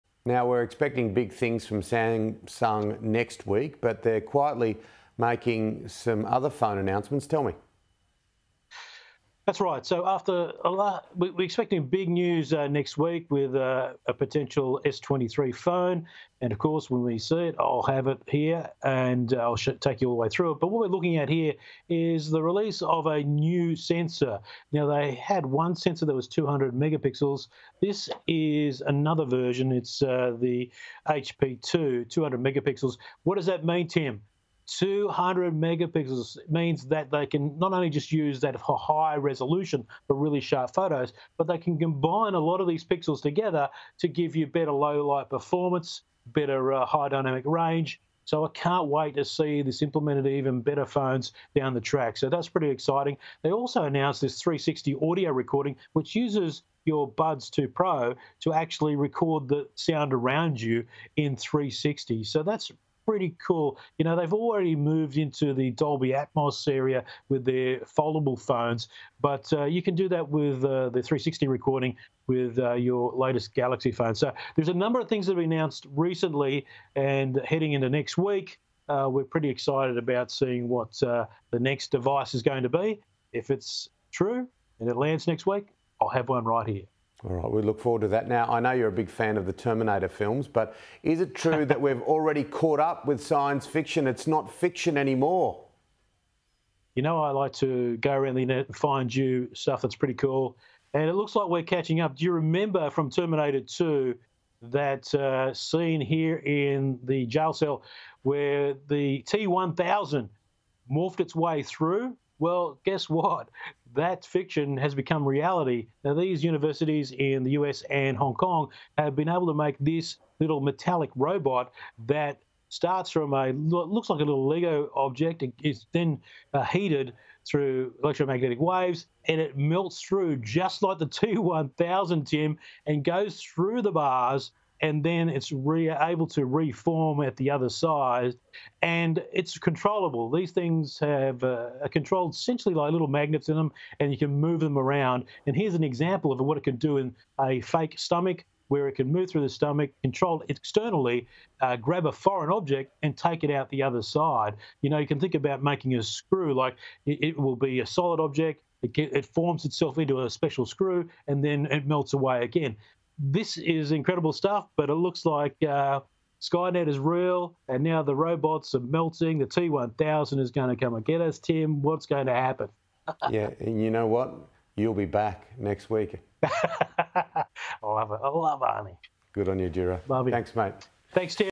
Samsung releases new 200MP camera sensor
Image Matrix Tech Editor Djuro Sen says Samsung's new 200-megapixel camera sensor can give better low-light performance and high dynamic range.

“Now they had one sensor that was 200 megapixels – this is another version, it’s the HP2 200 megapixels," Mr Sen told Sky News Australia.

“It means that they can not only just use that for high resolution for really sharp photos, but they can combine a lot of these pixels together to give you better low-light performance, better high dynamic range."